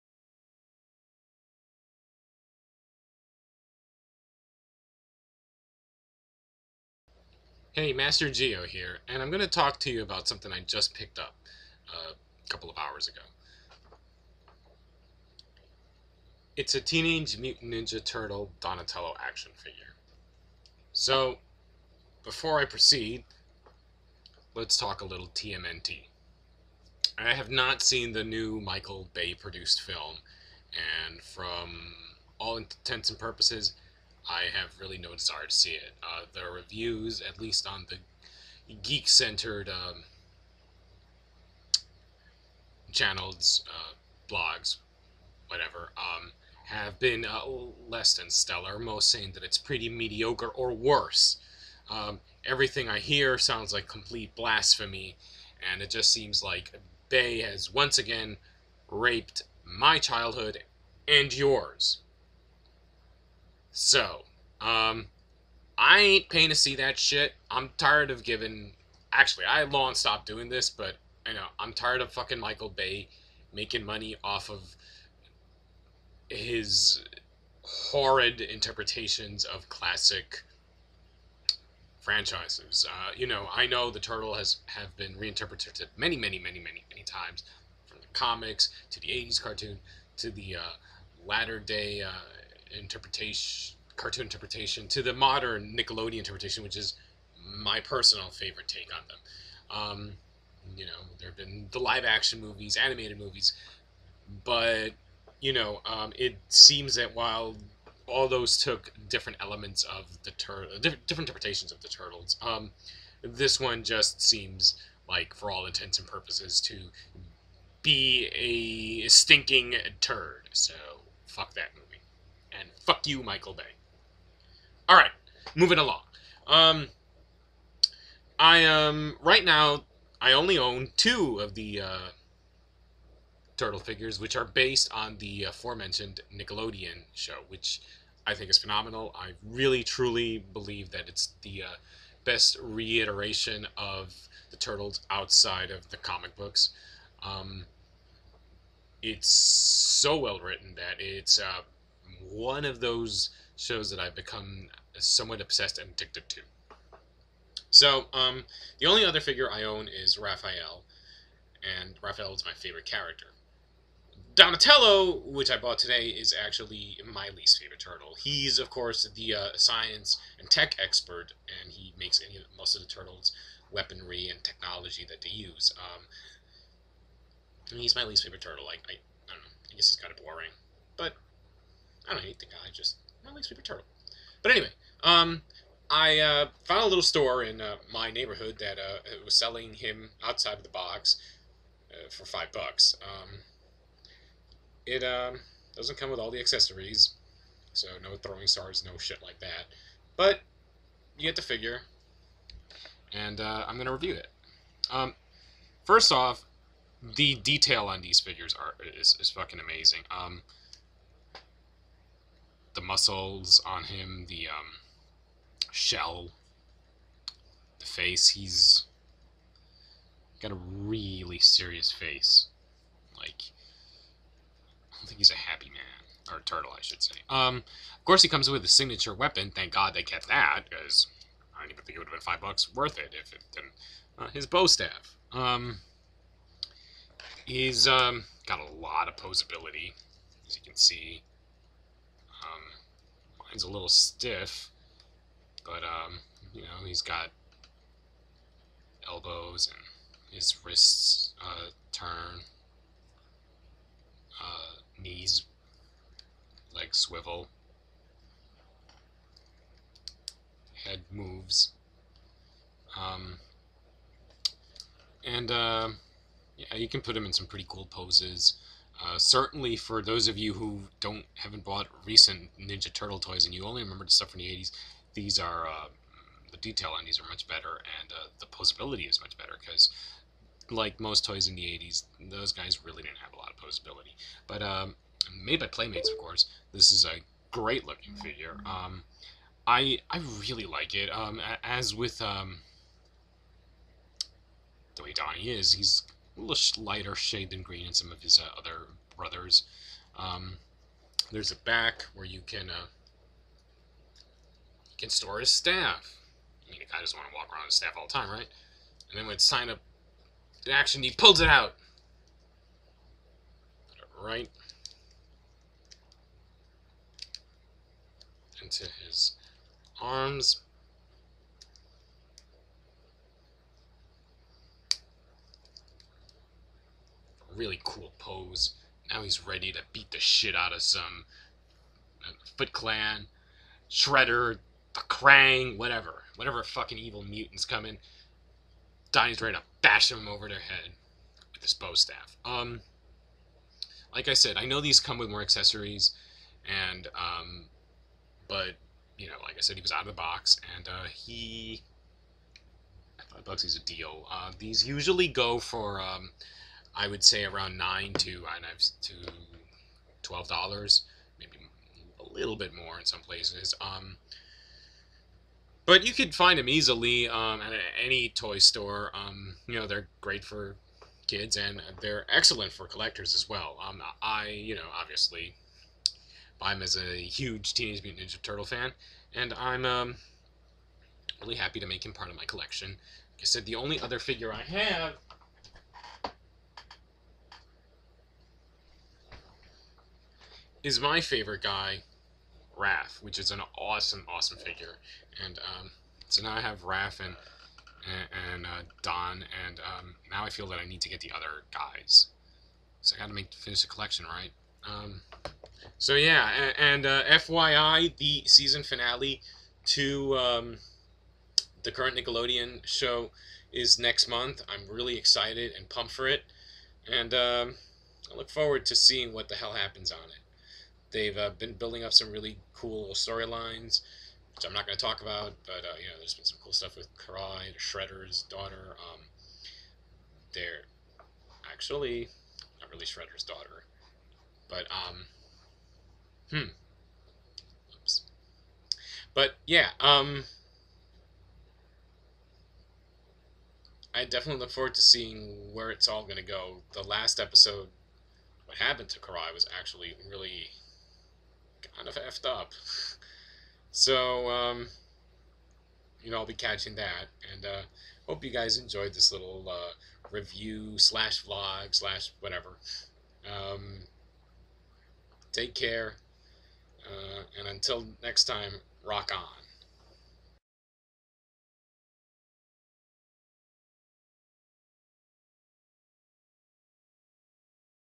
Nickelodeon TMNT Donatello Action Figure Review
I review an out of the box Donnie figure from the awesome Nick series